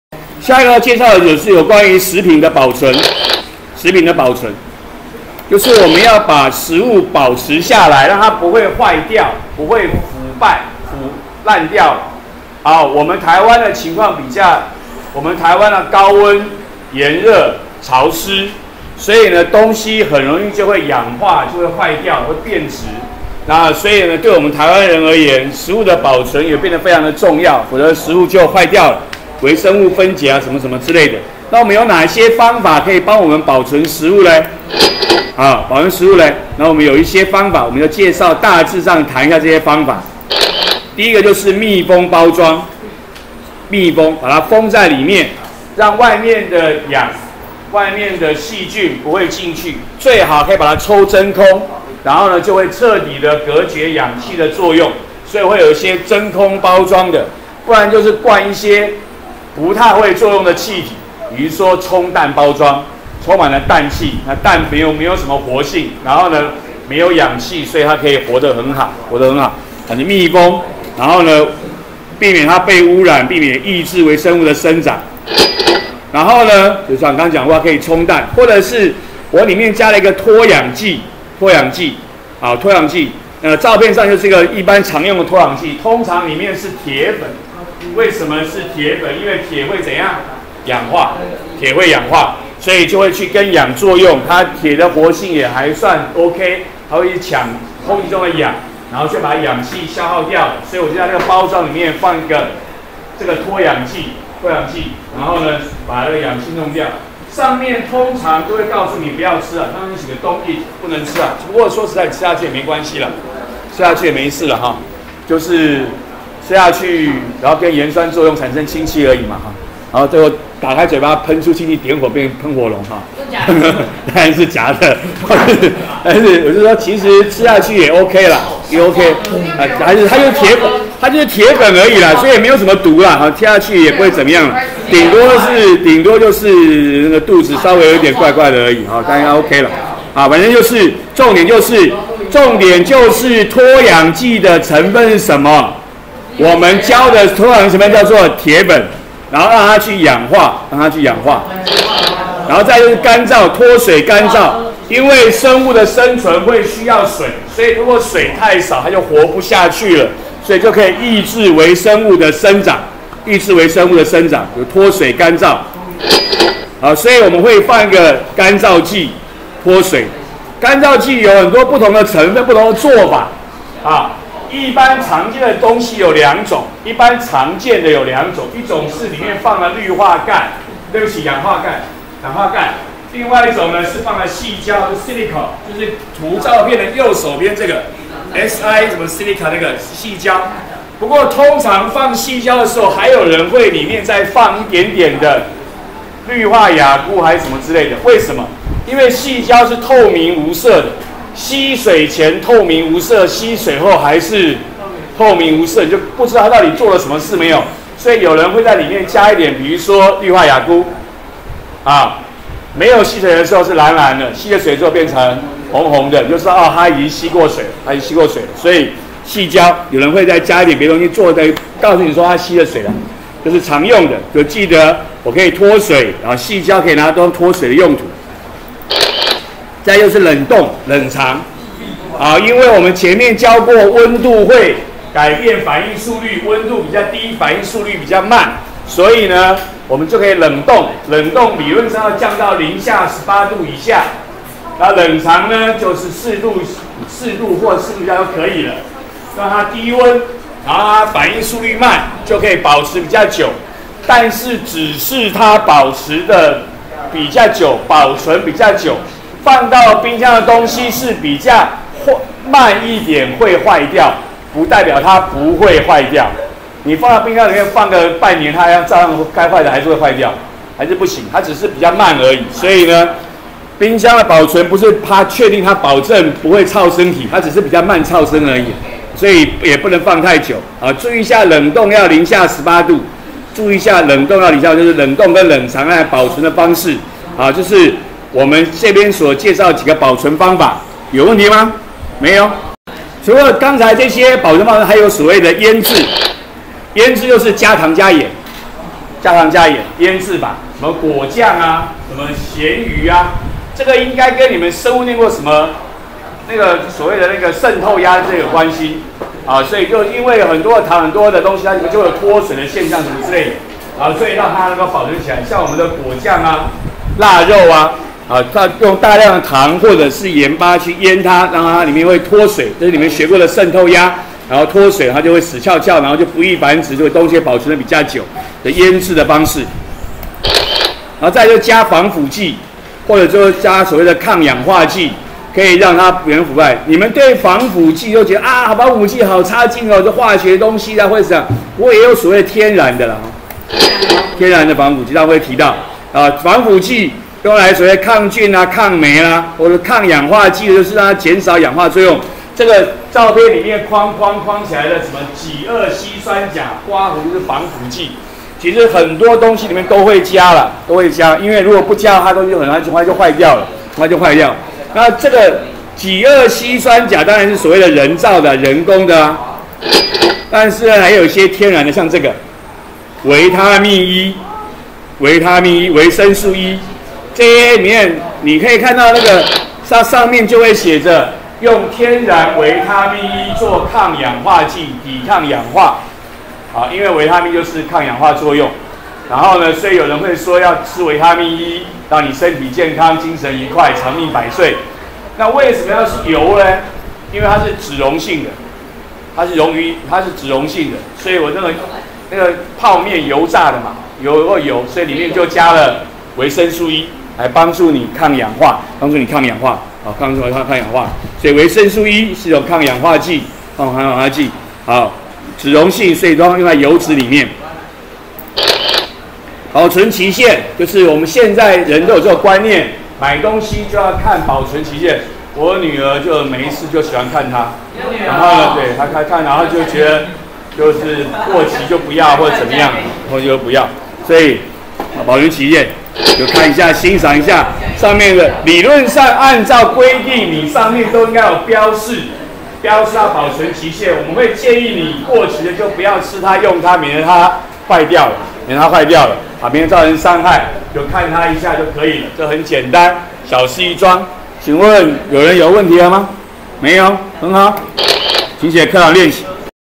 102學年國二理化_有機化合物17：食品保存
介紹食品保存的方法：密封包裝、脫水乾燥、冷凍冷藏、醃漬、防腐劑、抗氧化劑，說明保存期限。